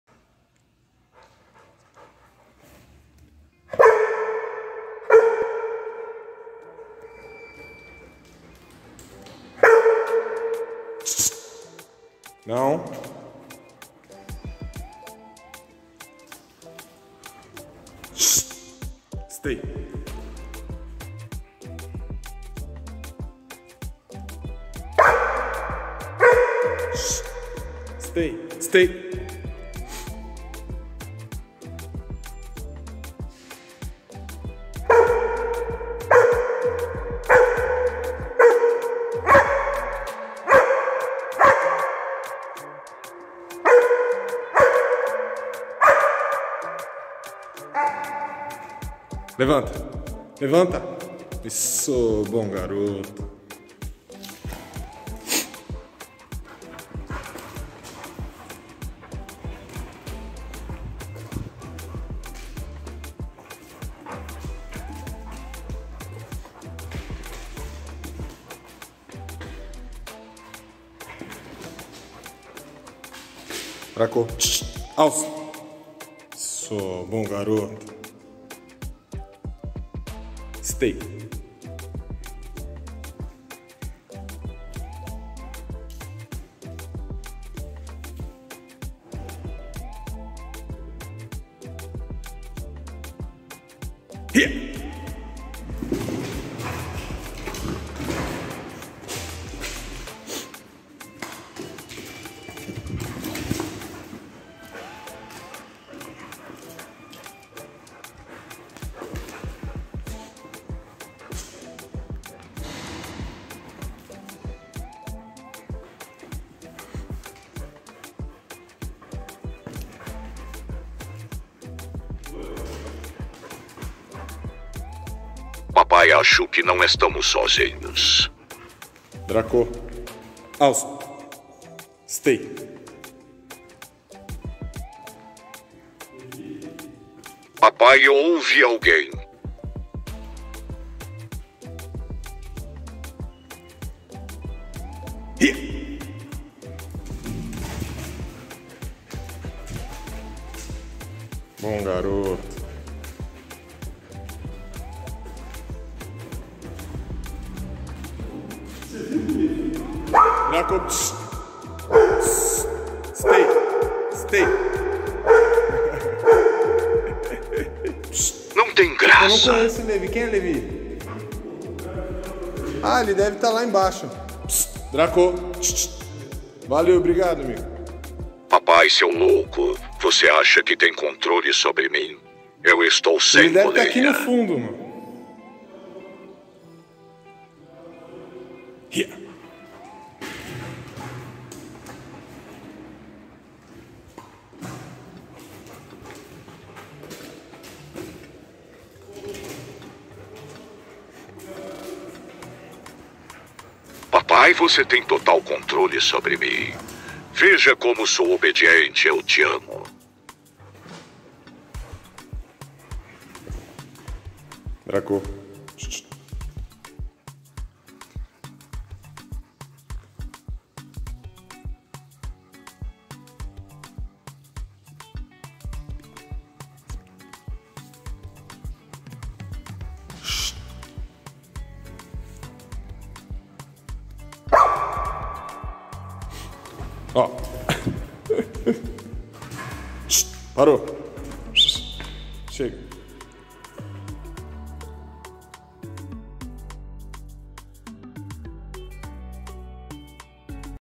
Como cansar um Pit Bull Final de rolê com Dracolino
Como cansar um Pit Bull
Andar 4 km kkkkk depois brincar de cabo de guerra